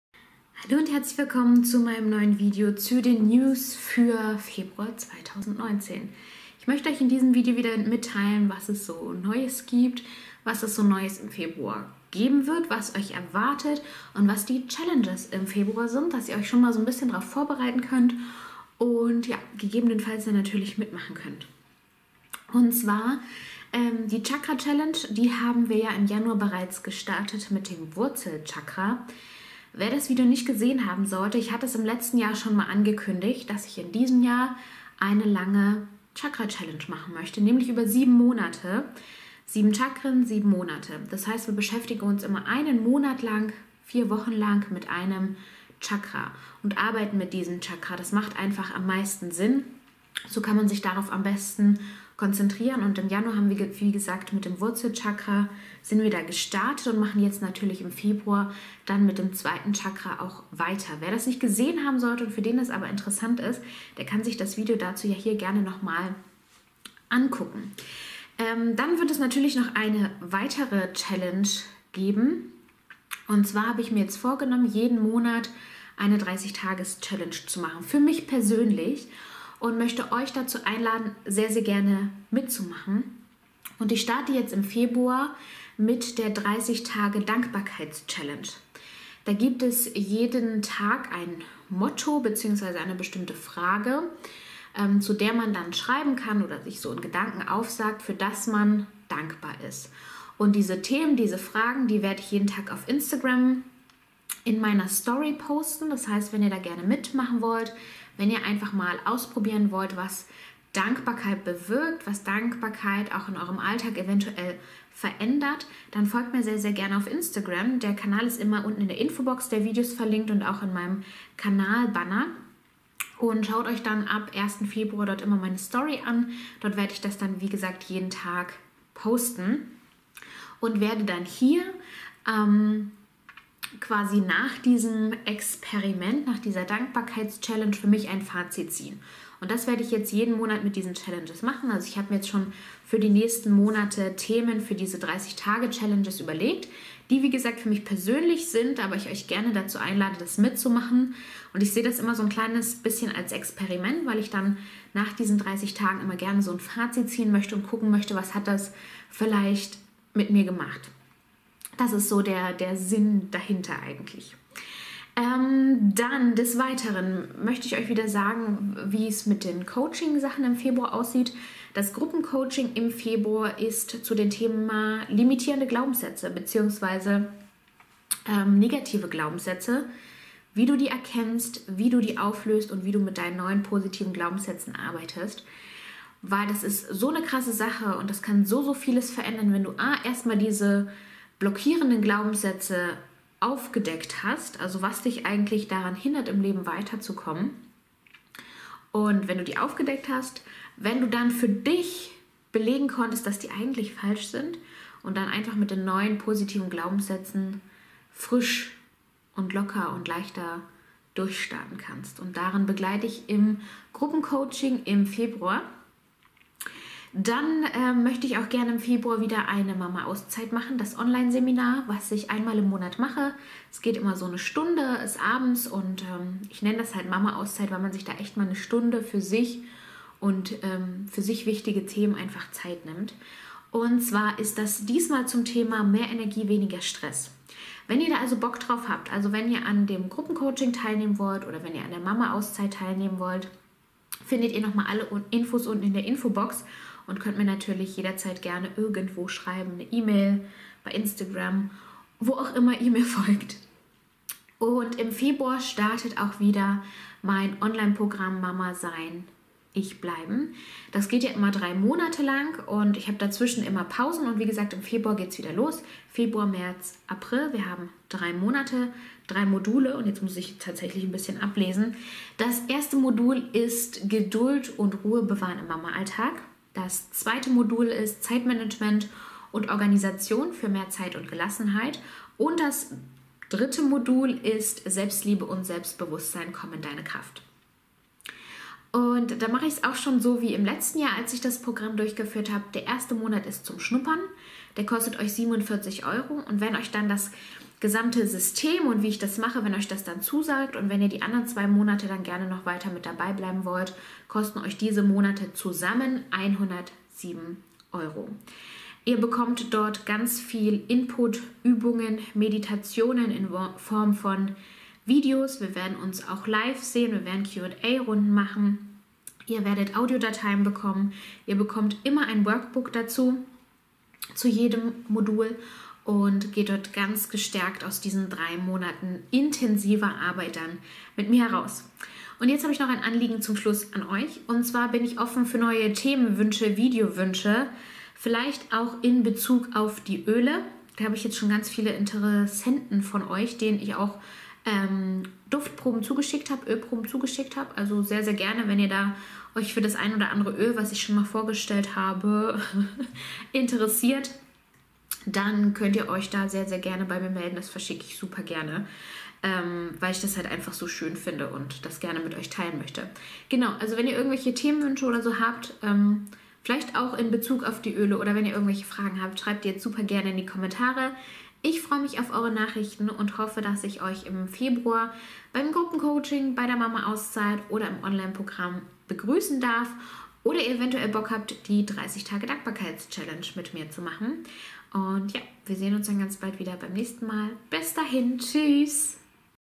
News, Challenges, Aktionen | farbenfrohe News Februar 2019 | Frau Farbenfroh
Was gibt´s Neues?

Gruppen Coaching im Februar: Blockierende Glaubenssätze aufdecken und auflösen - meld dich noch schnell an

Mama Auszeit im Februar: Weniger Stress, mehr Energie

Wenn ihr uns etwas zuschicken möchtet, dann bitte an diese Adresse:

Julia Ressel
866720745
Packstation 113
02906 Niesky